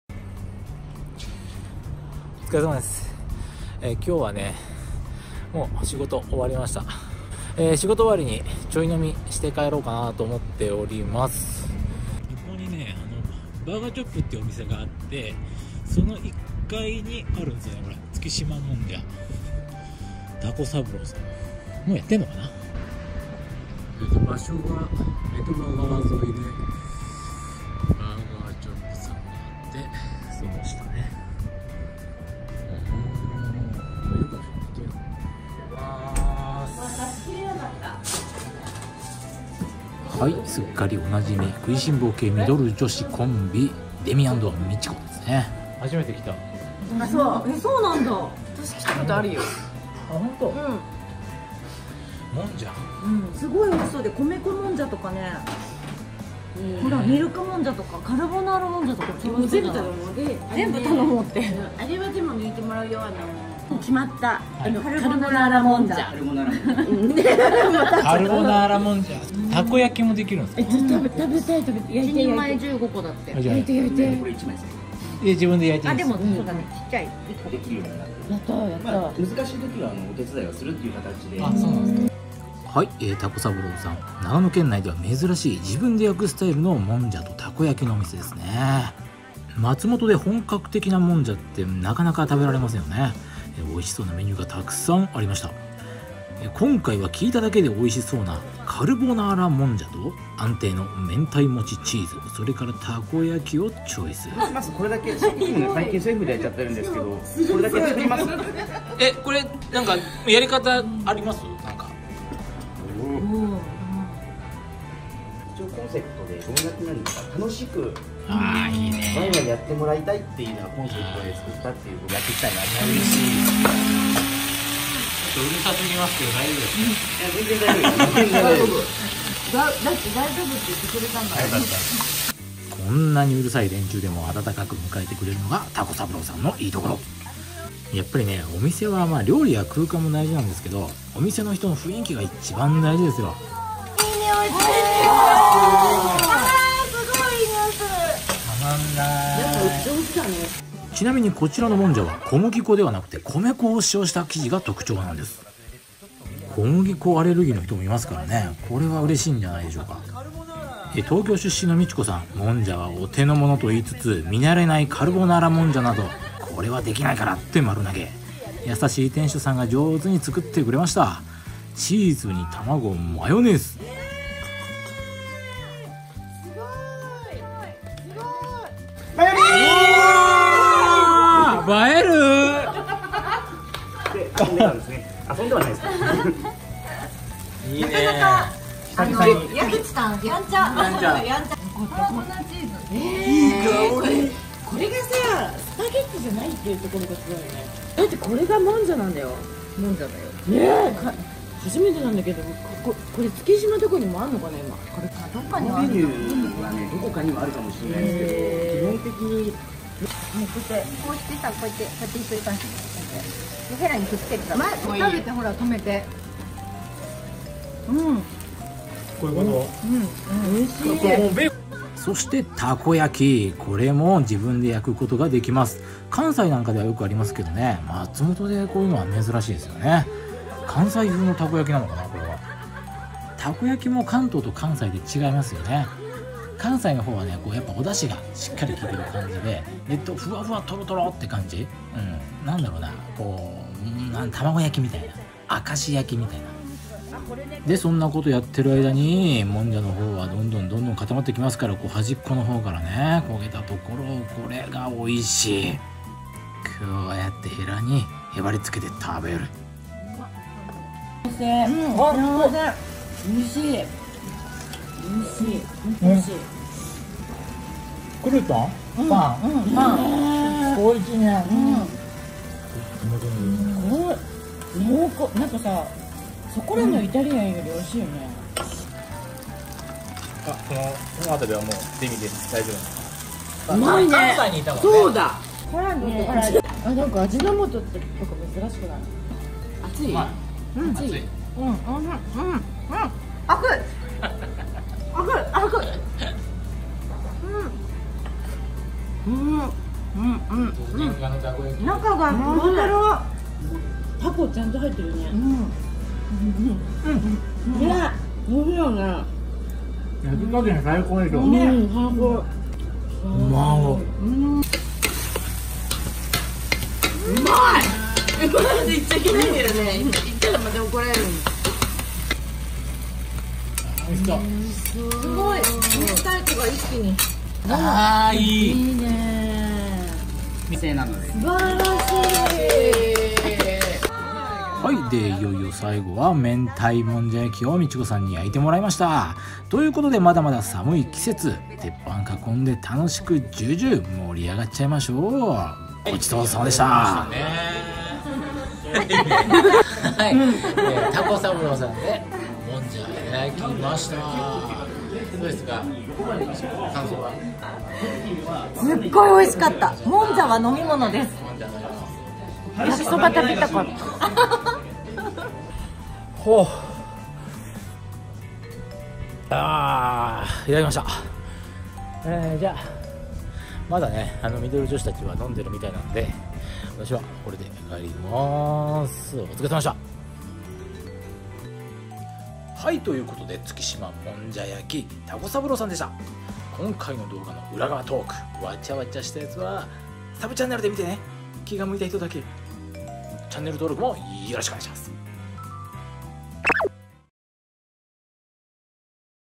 もんじゃ焼きとたこ焼きでチョイ飲み！ミドル女子も唸る激ウマもんじゃ焼き
おっさリーマンこの日はノー残業デー。

といっても別に会社で決まっているわけではなく、勝手に自分でそう決めただけですが…。

会社近くを流れる女鳥羽川沿いにある月島もんじゃのお店で、例の食いしん坊系ミドル女子コンビ

「デミ＆ミチコ」が飲んでいるとかいないとか…。

「蛸三朗」さんはハンバーガーグリルで有名なバーガーチョップさんの下にあります。

長野県内では、珍しい自分で焼くスタイルのもんじゃとたこ焼きのお店。

もんじゃは小麦粉ではなく米粉を使用した生地が特徴なんです。

本格的なもんじゃって松本ではなかなか食べられませんよね。

おいしそうなメニューがたくさんあります。

【お店の情報】
月島もんじゃ蛸三朗（タコサブロウ）
住所／長野県松本市大手2-2-4
※2019年3月22日時点の動画のため、情報が異なる場合がありますがご了承ください。

【食べたもの】
・カルボナーラもんじゃ　1200円
など

■取材のご依頼もお待ちしております。